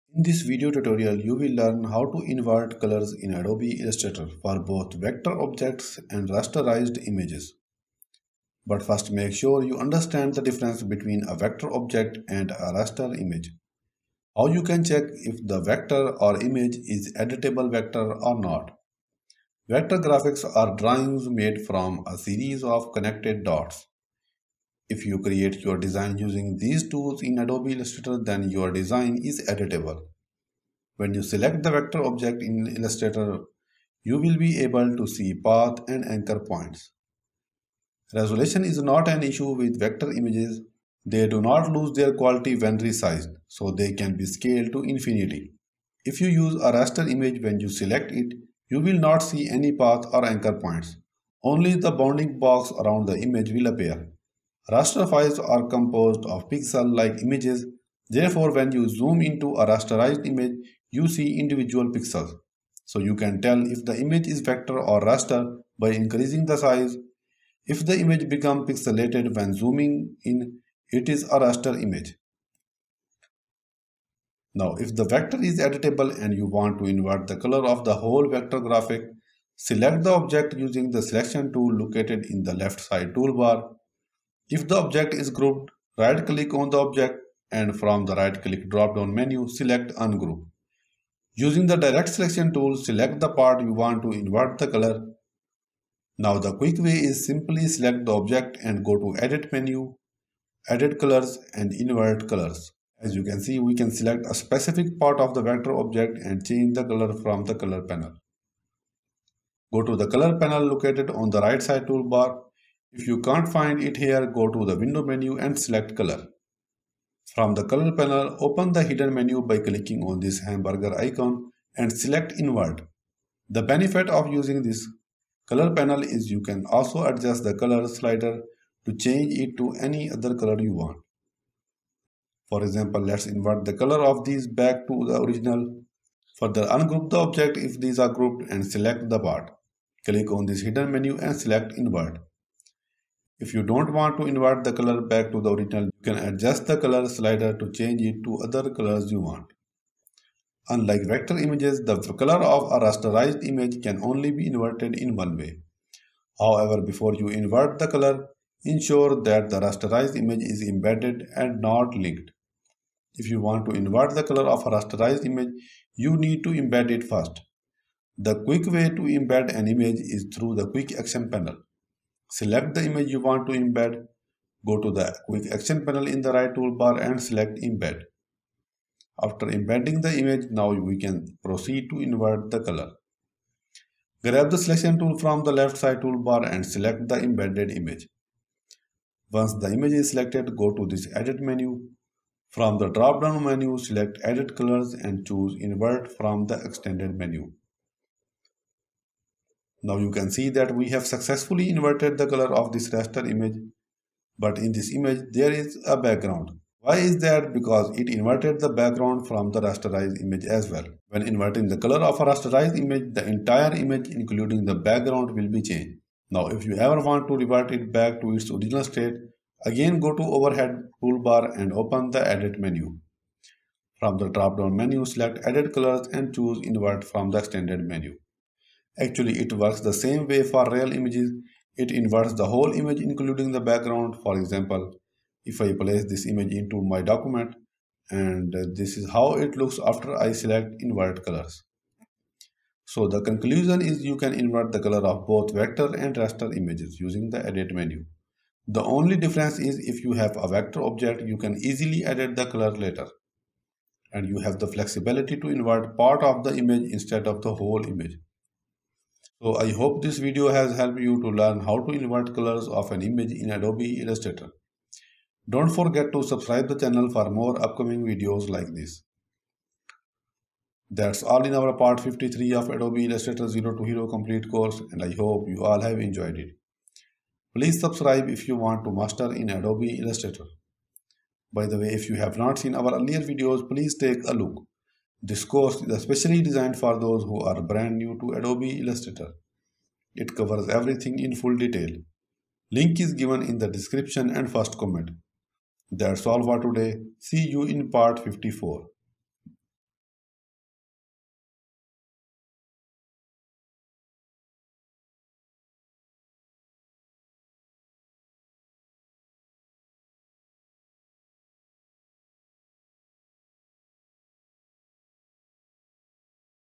How to INVERT Colors in Adobe Illustrator! (Using Vector Objects & also Images)
In this Adobe Illustrator tutorial video, you will learn how to invert colors in Illustrator! You can easily invert color in Adobe Illustrator for vector objects and rasterized images using the simple techniques mentioned in this video tutorial. Inverting color in Illustrator is a simple process that can make cool images and shape effects.

In Adobe Illustrator, you can use the Invert Colors option to invert any color of an image or object, including shapes, text, photographs, and different gradient shapes. In this video tutorial, you will learn how to invert colors in Adobe Illustrator for both vector objects and rasterized images. But also make sure you understand the difference between a vector object and a raster image. How you can check if the vector or image is an editable vector or not. I have explained all these types in this video tutorial.

Chapters:
0:00 Intro of Invert Colors in Adobe Illustrator
0:08 Difference Between a Vector Object and a Raster Image
0:19 Vector Objects/Graphics Explained!
0:45 Raster Based Images Explained!
01:14 How to Invert the Color of a Vector Object
01:42 Invert Specific Part of the Vector Object
01:52 Open Color Panel in Illustrator
02:03 Benefits of using Color Panel in Illustrator
02:30 How to Invert the Color of a Raster Image
02:43 IMPORTANT! Embed Raster Images First
02:52 How to Embed Raster Images in Adobe Illustrator
03:24 Issue with Raster-based Images in Adobe Illustrator
03:35 How to revert back the color inversion in Illustrator
04:02 My Final Thoughts on Inverting Colors in Illustrator

Welcome to our Adobe Illustrator tutorial on how to invert colors! In this video tutorial, we'll guide you through the process of inverting colors using both vector objects and raster images. Whether you're a beginner or an experienced designer, mastering this technique will take your designs to the next level. Watch now and unlock the secrets of Adobe Illustrator color inversion!